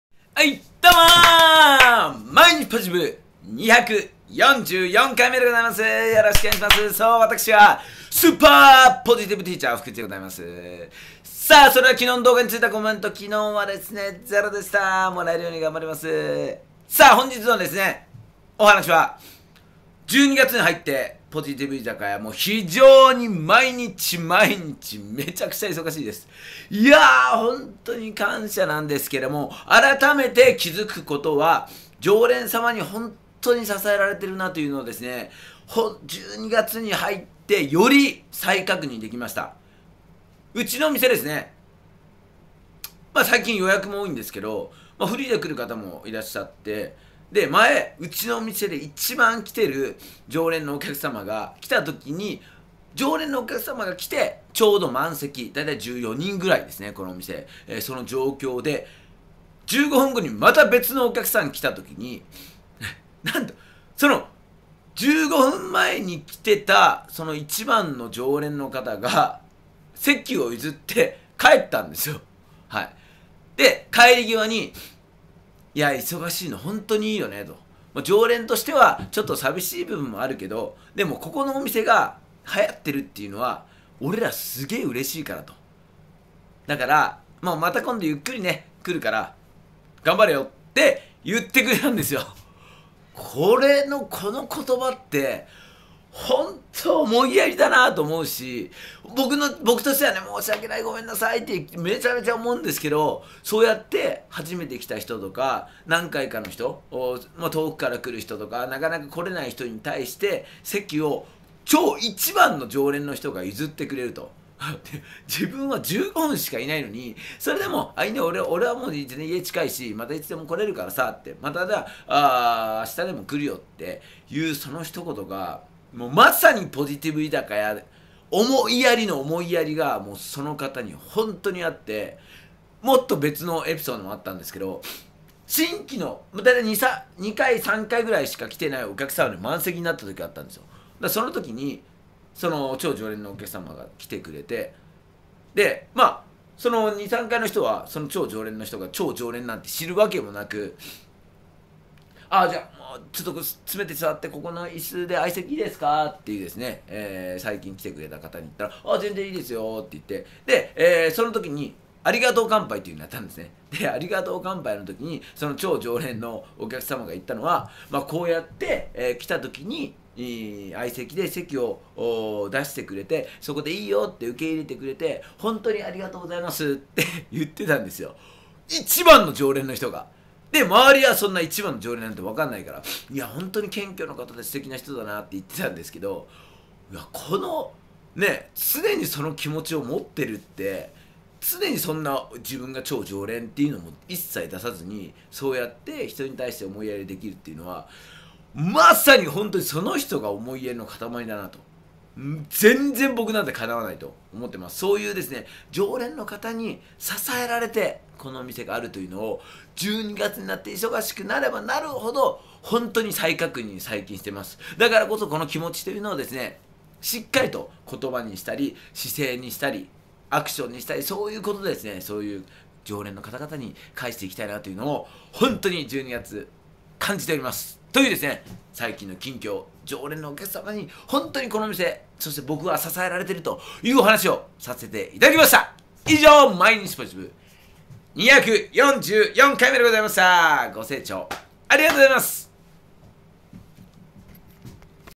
(244)毎日ポジティブ12/13 常連のお客様に支えられている
12月に入り
ポジティブ居酒屋連日
多くのお客様に来て頂いております
本当に感謝です(*^^*)
その中で再認識出来た事を
シェア致します✨